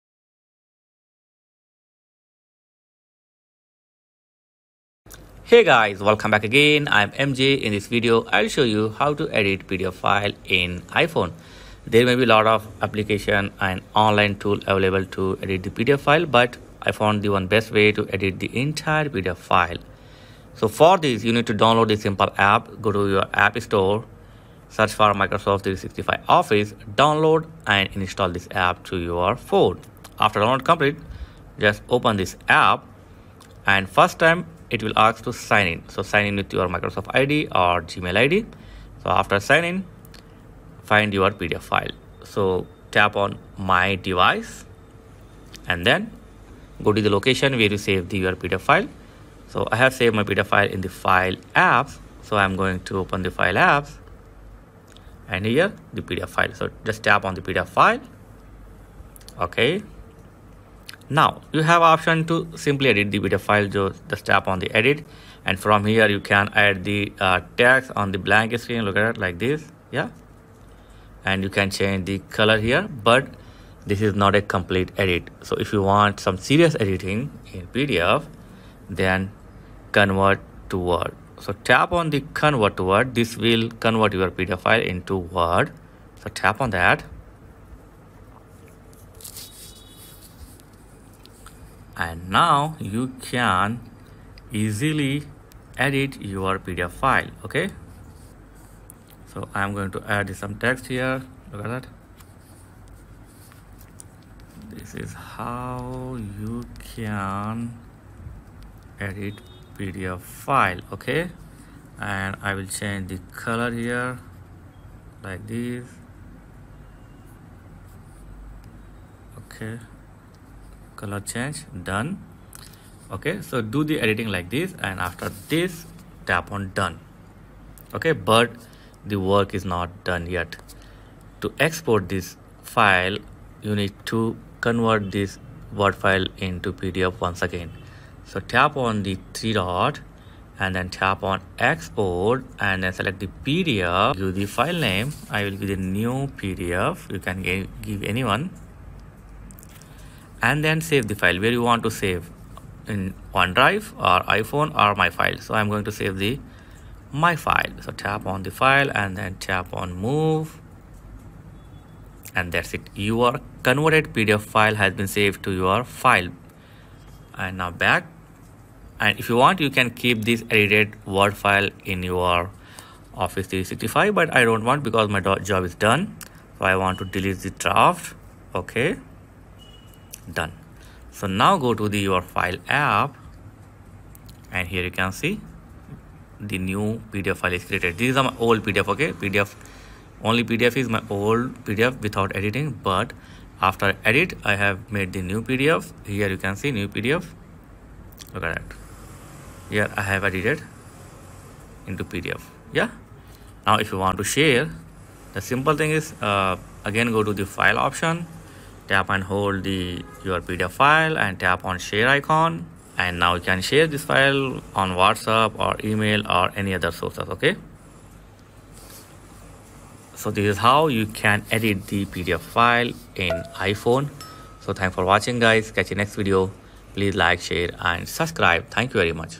Best Free Way to Edit PDF File on iPhone (Free PDF Editor for iPhone)
Learn best free way to edit PDF files on your iPhone without spending a dime. In this video, I show you step-by-step guide to how to edit PDFs using free apps in iPhone. Whether you need to annotate, fill forms, or make quick edits.